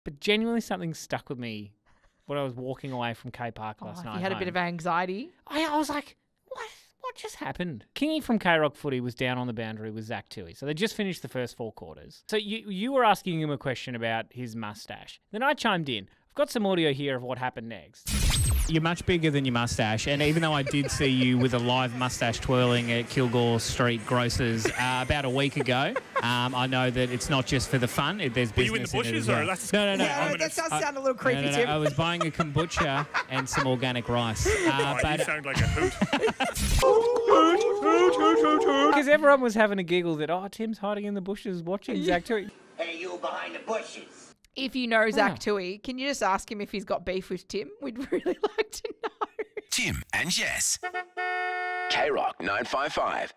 AFL player Zach Tuohy roasts radio host
Tim and Zach Touchy have serious beef